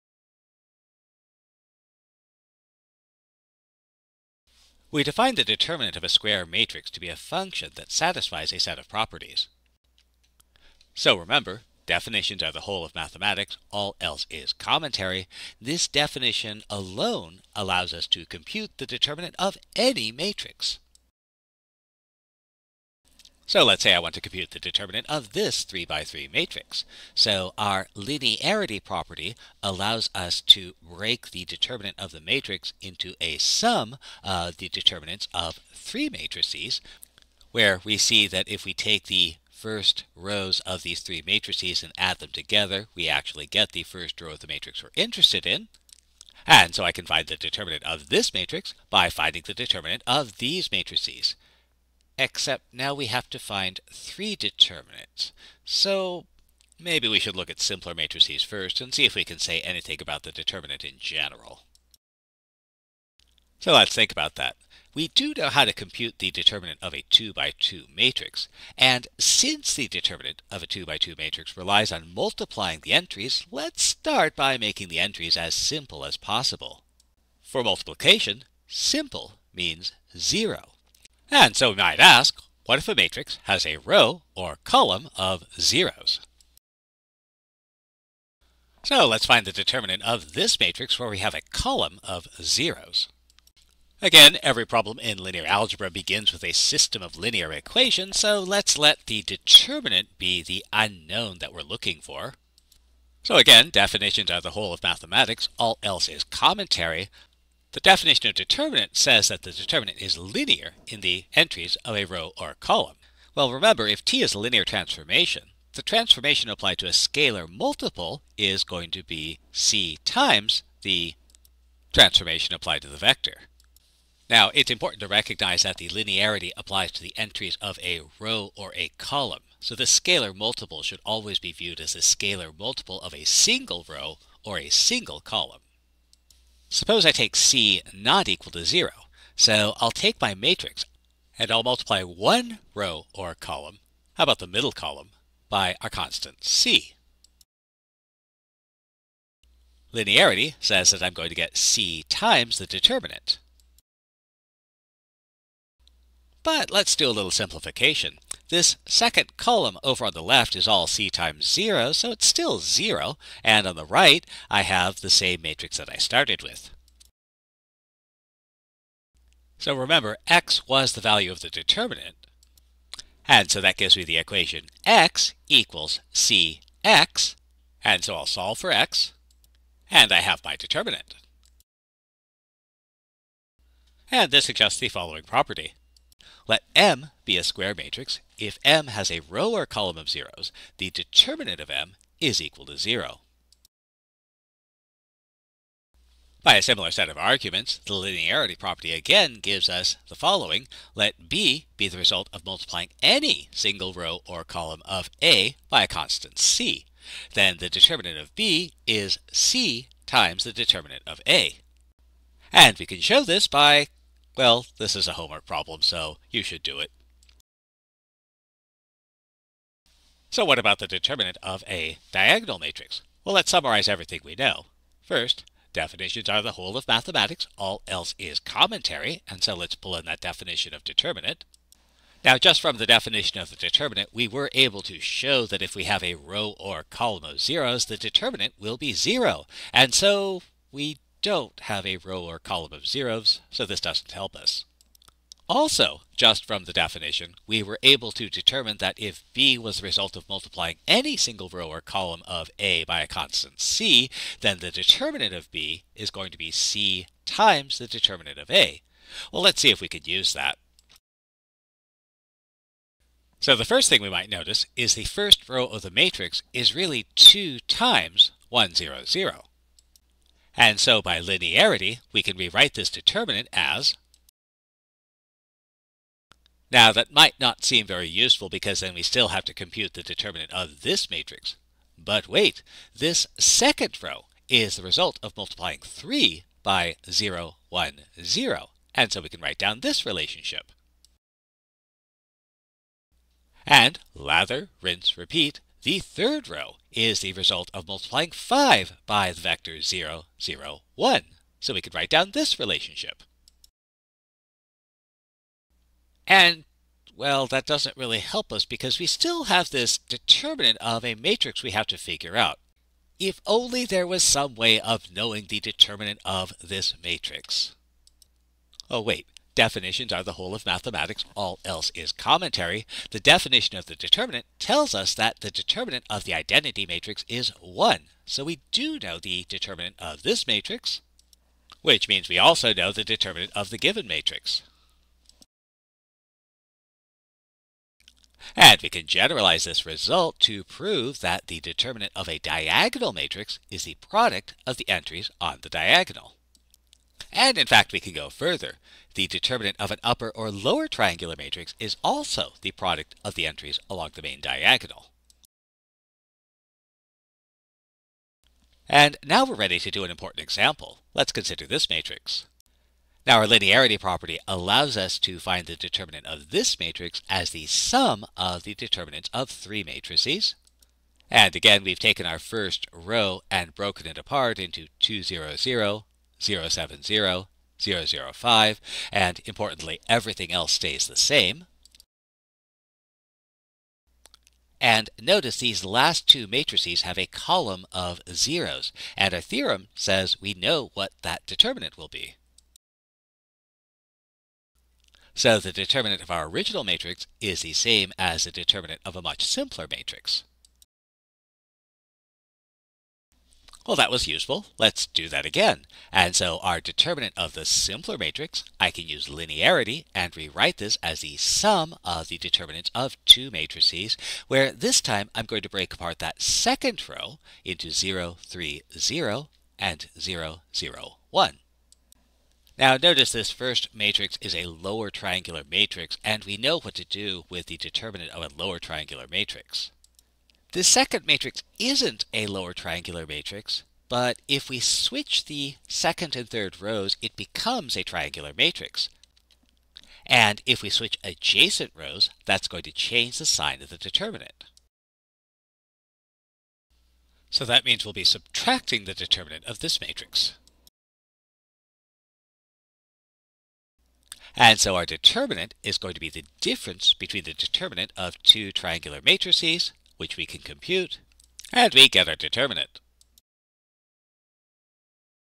Determinants, Part Two
More about determinants as a linear function of the rows and columns of a matrix.